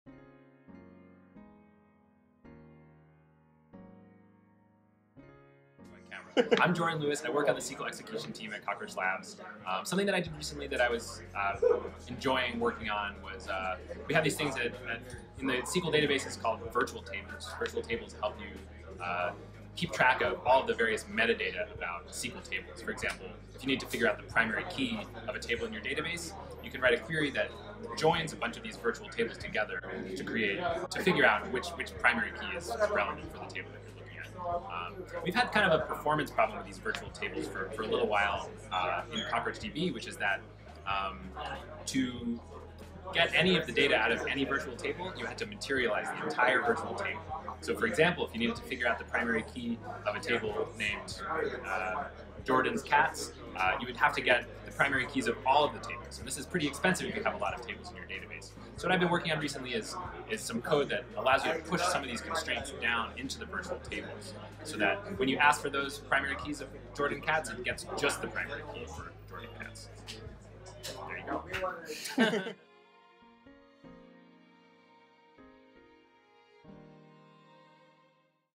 Roacher of the Week: Jordan Lewis, Engineering Manager at Cockroach Labs
This week's Roacher of the Week is Jordan "Large Data Bank" Lewis, an engineering manager at Cockroach Labs.

You may know him as "The Twitch Guy", or you may remember him from his early years as an Abercrombie & Fitch model, or possibly you bumped into him in a mosh pit at a Tool concert in the early aughts.

No matter where you know him from, you've no doubt sensed the overwhelming integrity and earnest desire for everyone to be happy and for truth/transparency to guide us through the growth of Cockroach Labs (and through life).